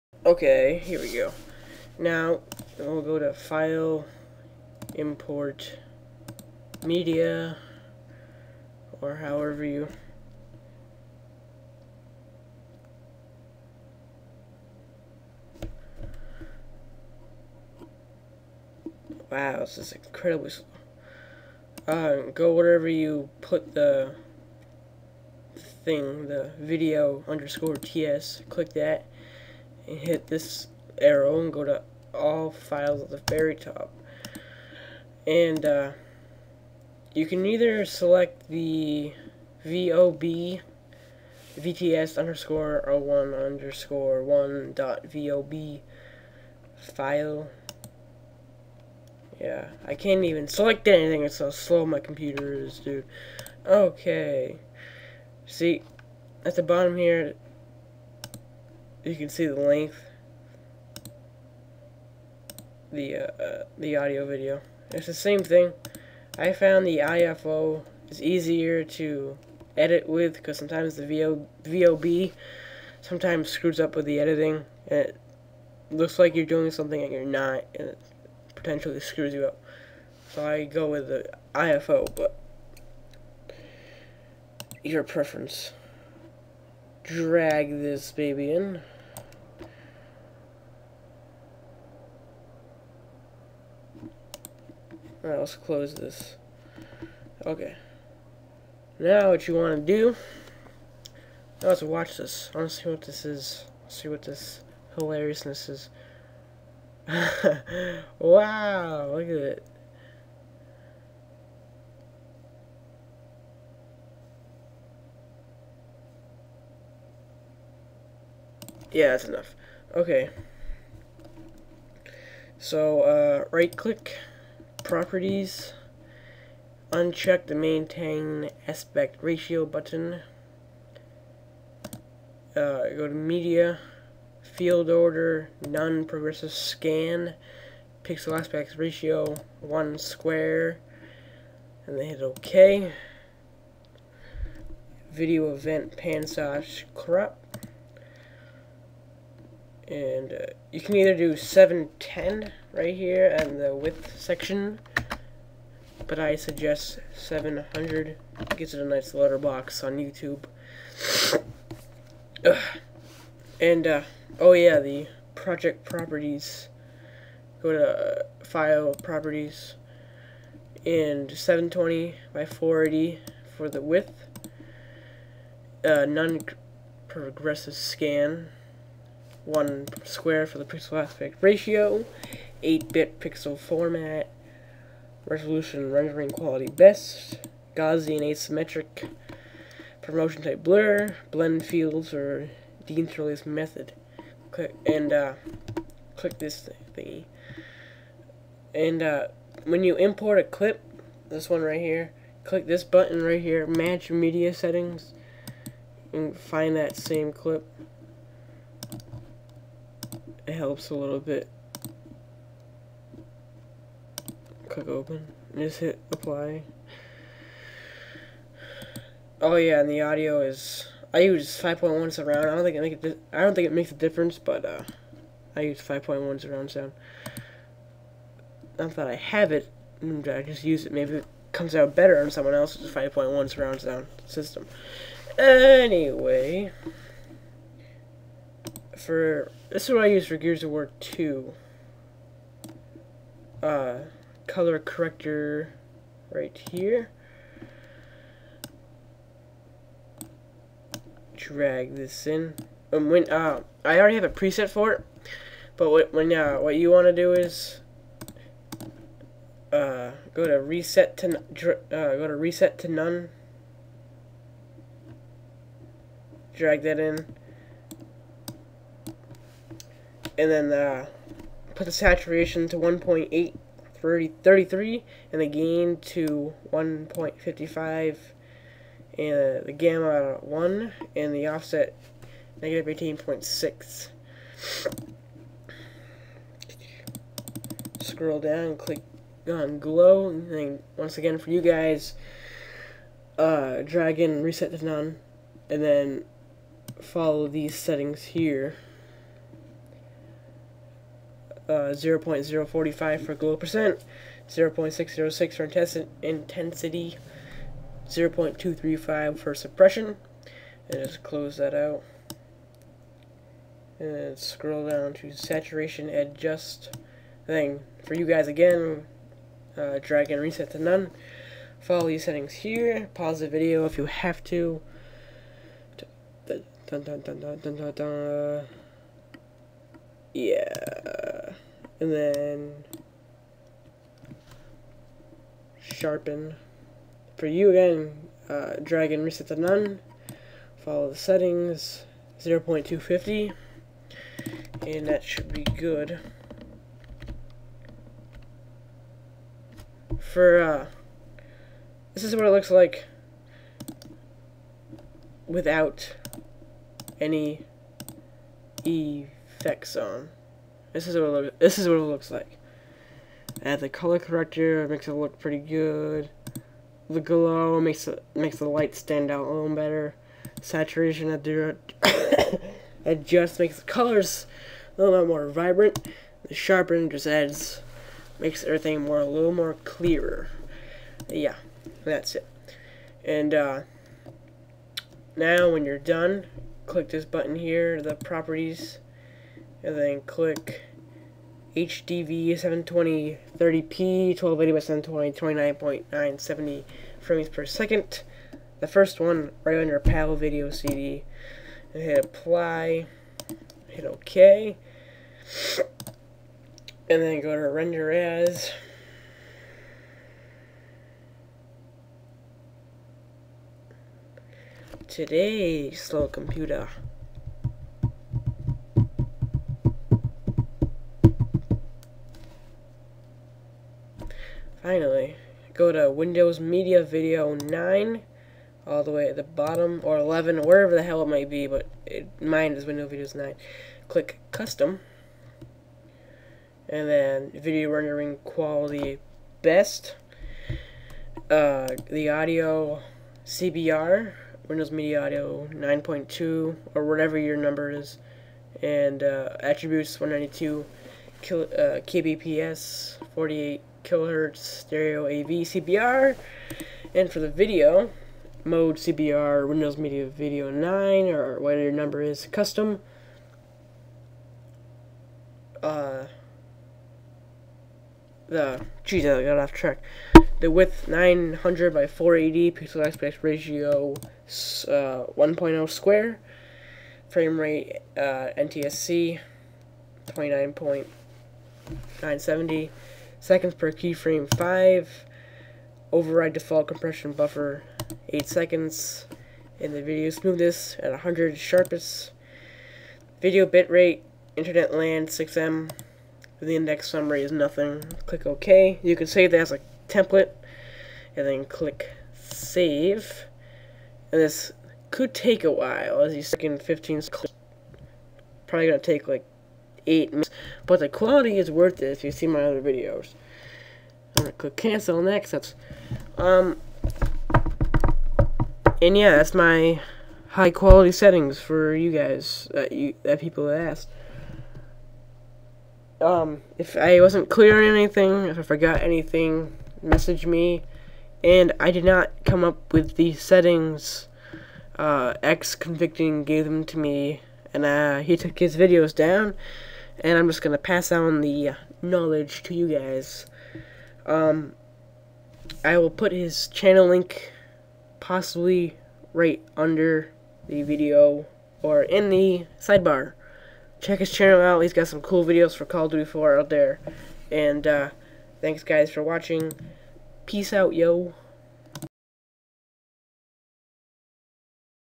Dazzle High Quality Settings(The Best Out There) Part Two
Thanks to xConvicting for the settings!

This is part two of how to get very good settings with a dazzle. I personally use the Dazzle DVC 100 or the Dazzle Video Creator Plus as it is known. But it doesn't matter. If you use Sony Vegas then this is perfect for you and if you use some other editing program this may puritan to you but I'm not 100% sure.

If I wasn't clear on some parts or if you just have any questions just PM me here on Youtube